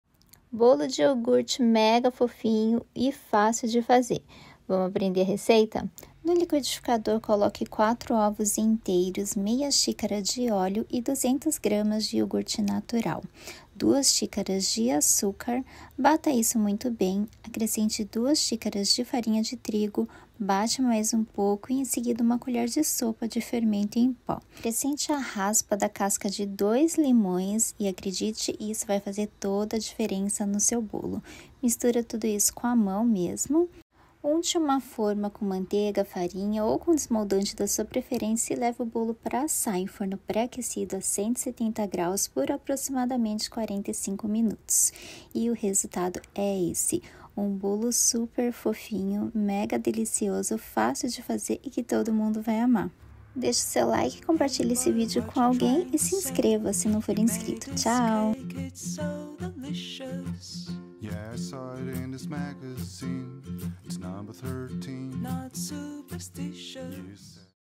Bolo de Iogurte (receita fácil e rápida)
Bolo de Iogurte

4 ovos (em temperatura ambiente) ou 200g
1/2 xícara de óleo ou 120g
200g de iogurte natural
2 xícaras de farinha de trigo ou 280g
2 xícaras de açúcar ou 320g
1 colher (sopa) de fermento em pó ou 15g
raspas da casca de 2 limões

Modo de Preparo:
Unte uma forma com manteiga e farinha ou com o desmoldante da sua preferência!
Preaqueça o forno a 170° graus (ou na temperatura que está acostumada assar bolos)
No liquidificador bata os ovos, o óleo, o iogurte e o açúcar. Acrescente a farinha de trigo e bata novamente. Por último acrescente o fermento em pó, bata mais um pouquinho e as raspas da casca do limão.
Leve para assar no forno pré-aquecido por
40~45 minutos
Desenforme morninho, quando já conseguir segurar a forma!